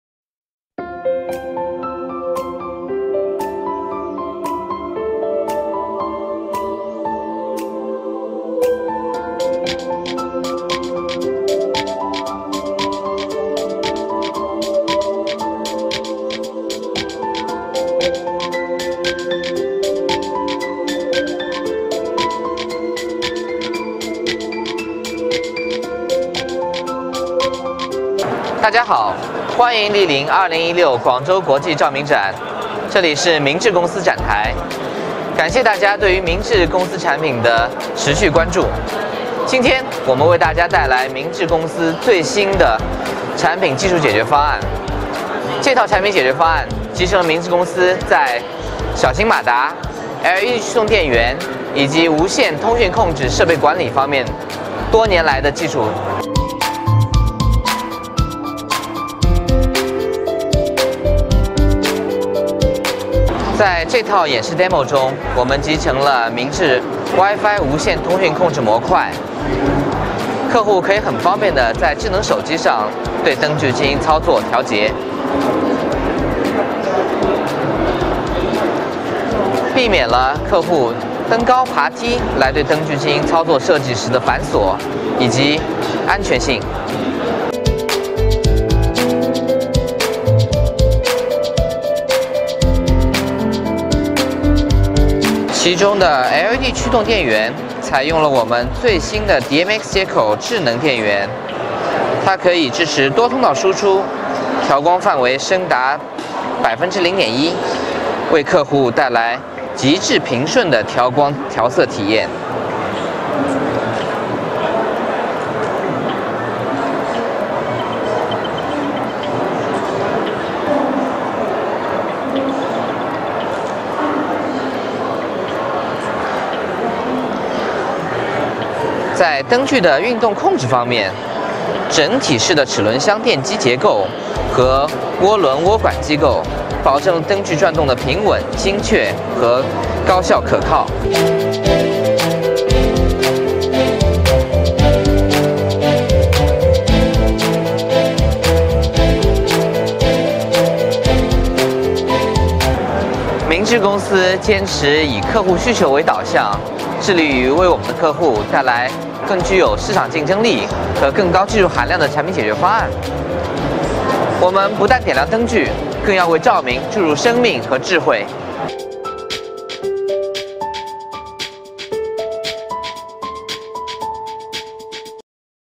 Guangzhou International Lighting Exhibition2016
See our latest products and technical solutions in 2016 Guangzhou International Lighting Exhibition
The latest products solution integrated Moons’ small motor, LED drive power and wireless control equipment management technology.